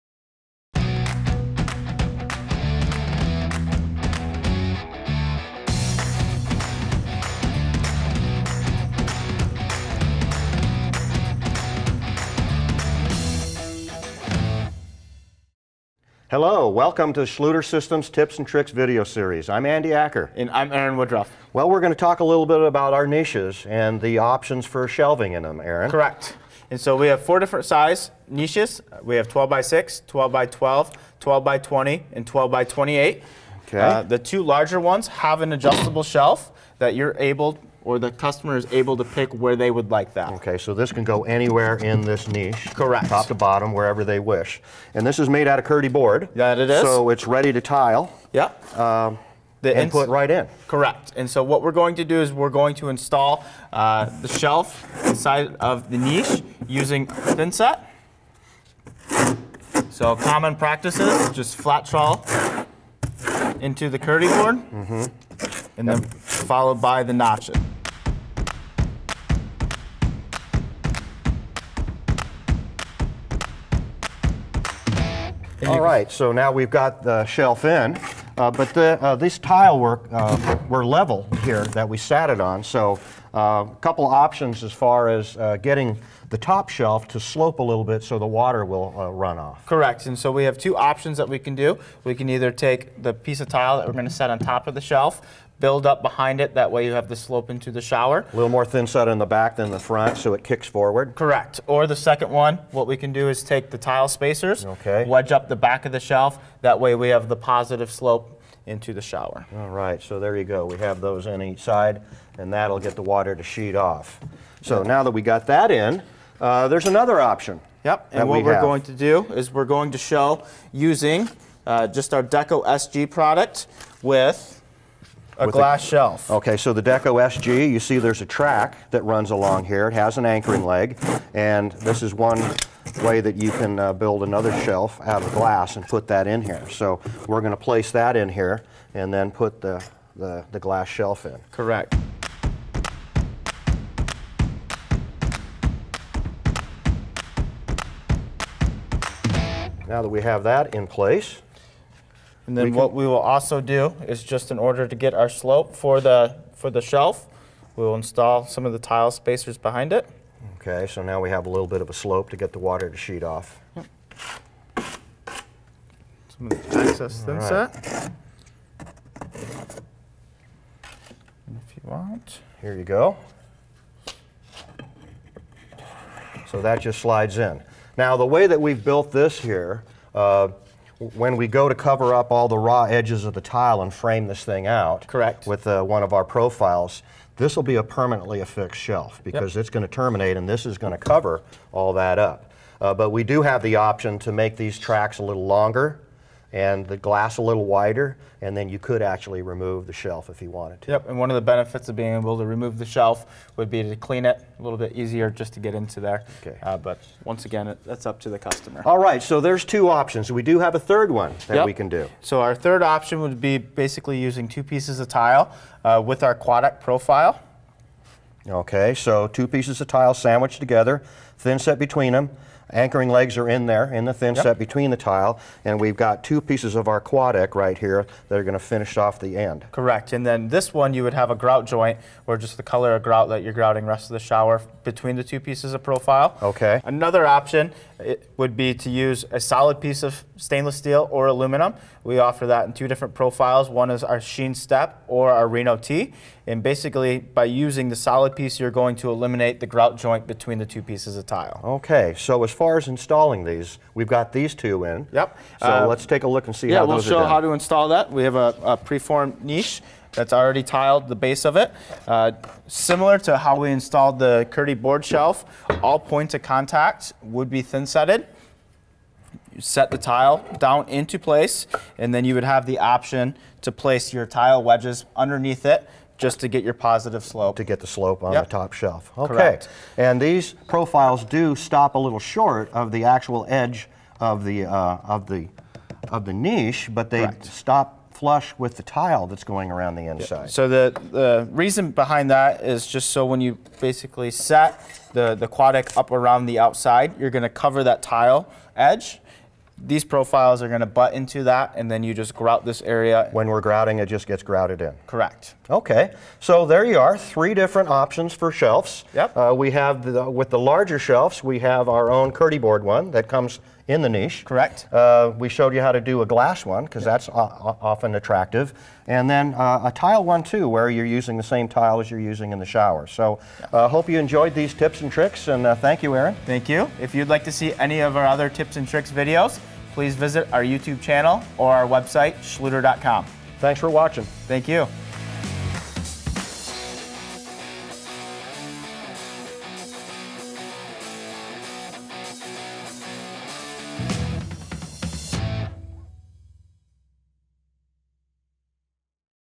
How to create a shelf in a niche
The KERDI-BOARD-SN prefabricated shower niches are available in many different sizes to suite your desired design. Some of the larger options come with pre-fabricated shelves for more storage in your shower. There are other options for creating shelves in our shower niches as well. Check out these tips on some different options for creating a shelf for your shower niche!